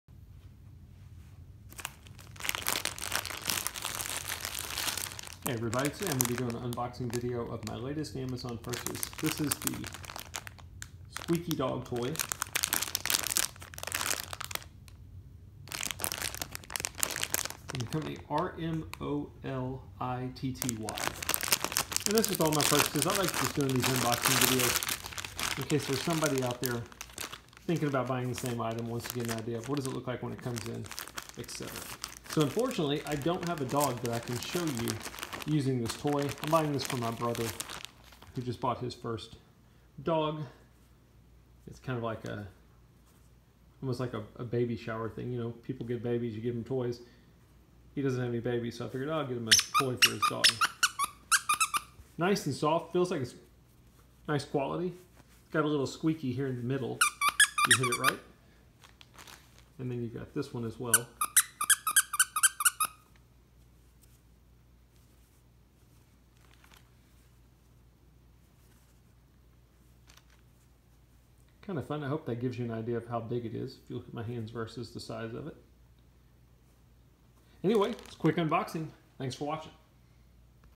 Rmolitty Dog Toy (2 Pack) - Unboxing- Bought on Amazon: )
This is an unboxing video of the Rmolitty Dog Toy (2 Pack).  More information can be found at the following Amazon